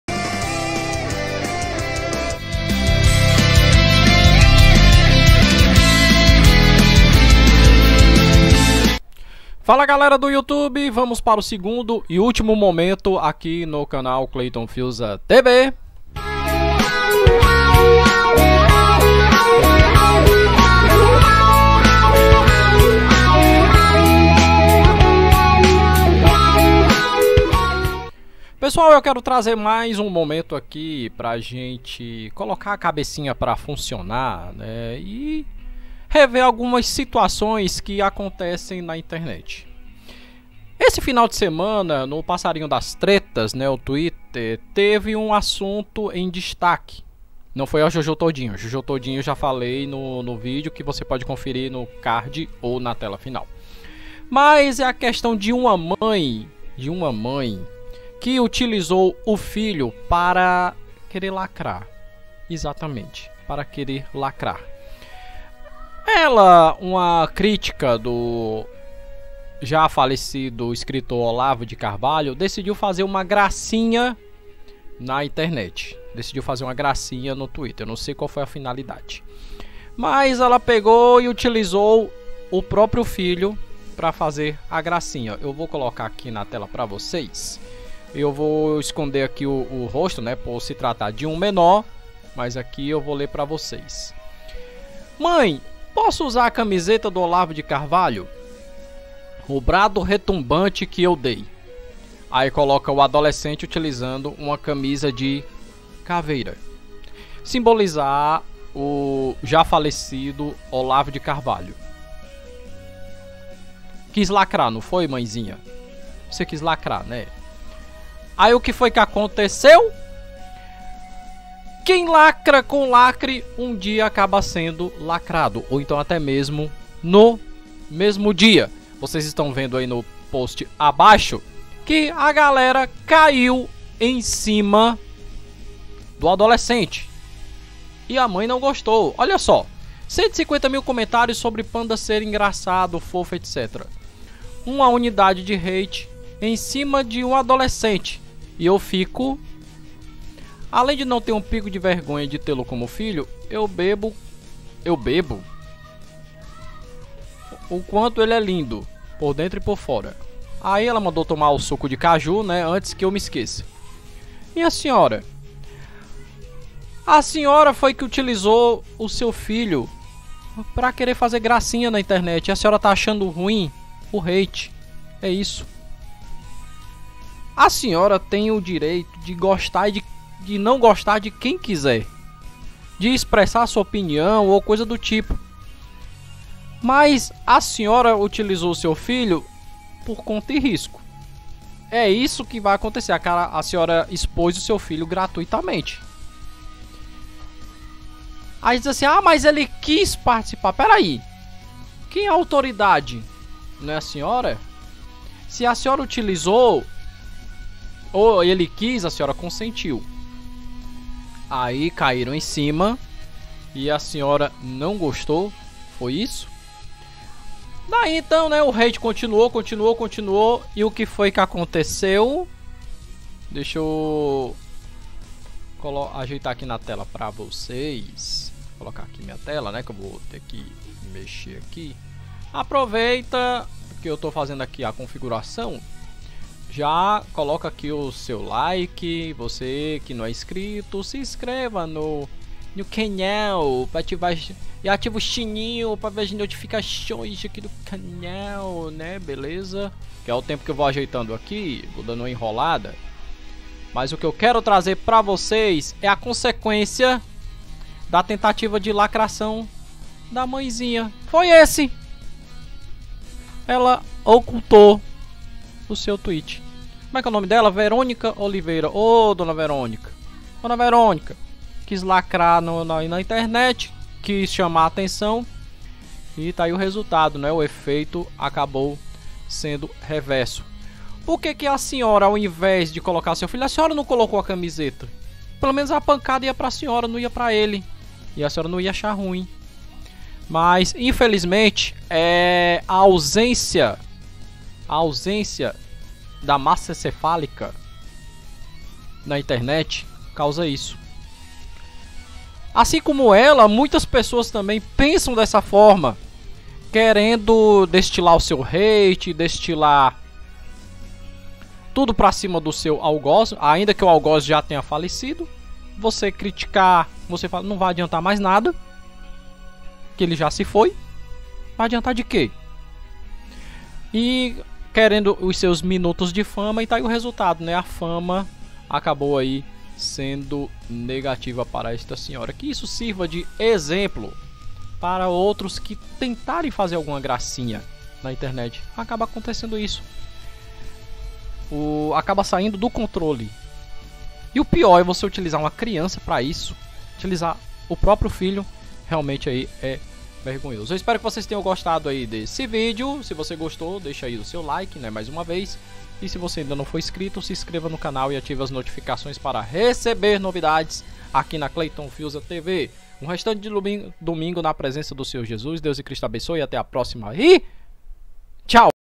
Mulher usa filho pra debochar de Olavo de Carvalho e...
O domingo foi agitado no Twitter que eu chamo de passarinho das tretas. Uma pessoa que é desafeta do escritor Olavo de Carvalho, falecido neste ano, decidiu fazer uma gracinha para seus seguidores na rede e o pior, usou o próprio filho para fazer a lacração.

Só que não esperava a reação das pessoas de forma negativa em cima do adolescente por conta da gracnha sem graça. A mãe acabou achando ruim da situação que ela mesmo criou e acabou bloqueado seu perfil.

É meus amigos, atitude irresponsável dela e ao mesmo tempo infantil.